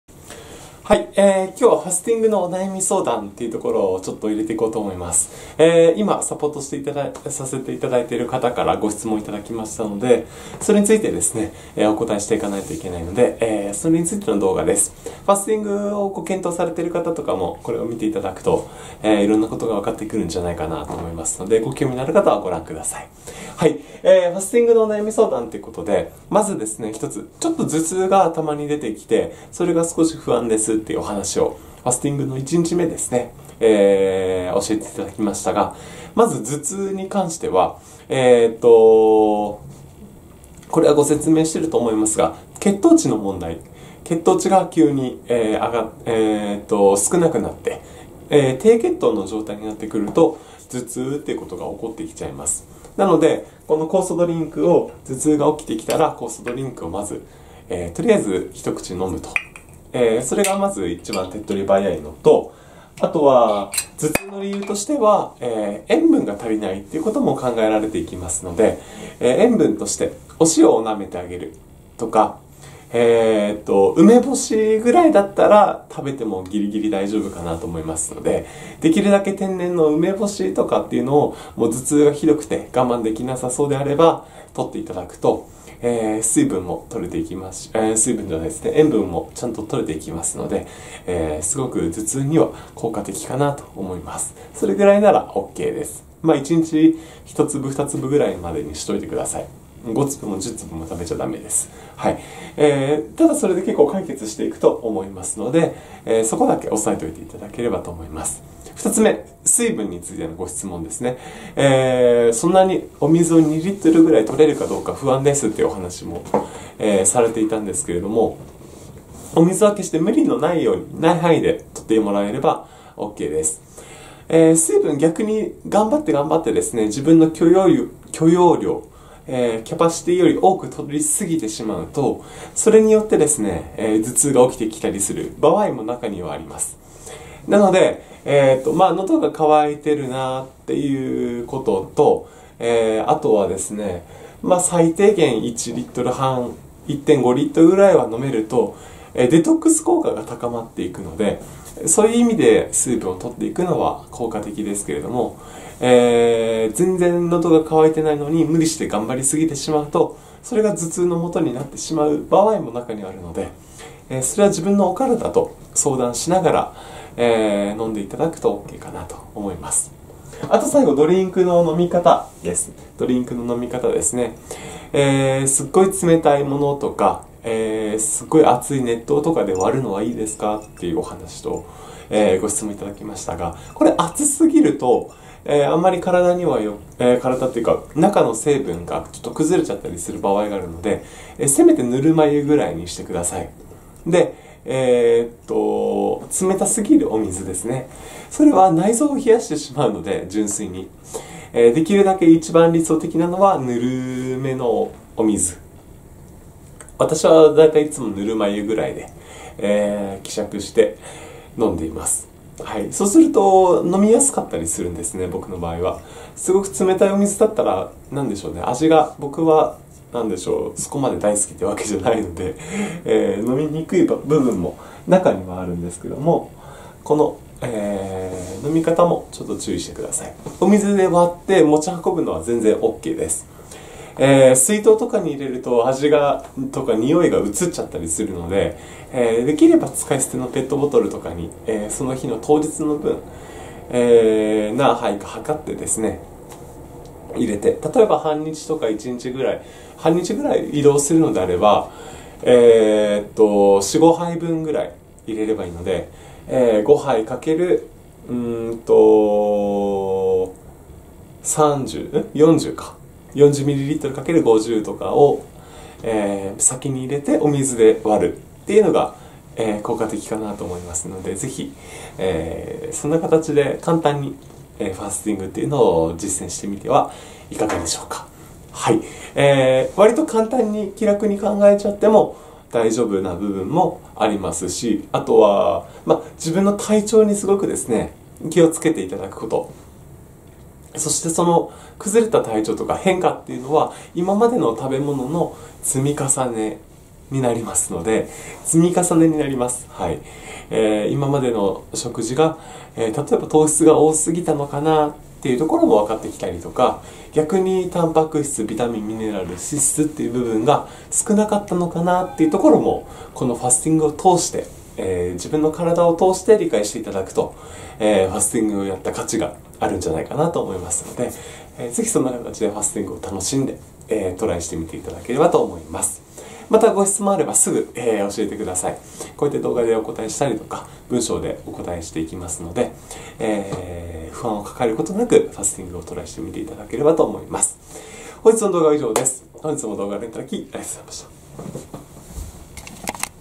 【ファスティングお悩み相談】頭痛・水分量・ドリンクの飲み方
1993年9月3日産まれ（ドラえもんと同じ笑）